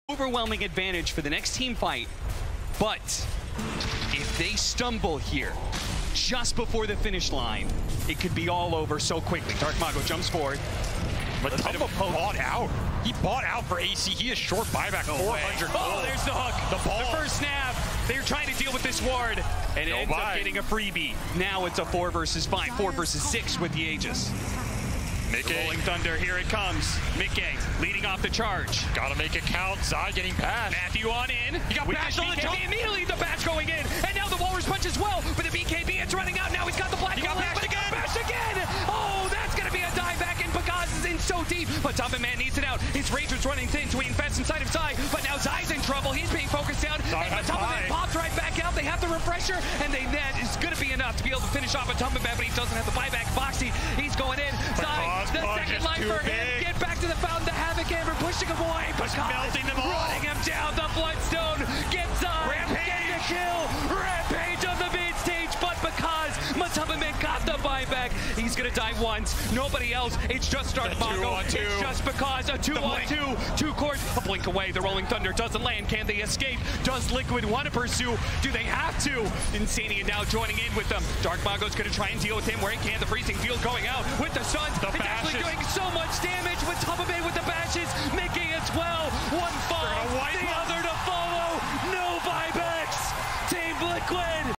TEAM LIQUID ARE DEFENDING THE ANCIENT
The moment team liquid successfully defending their base from Thunder Awaken final push.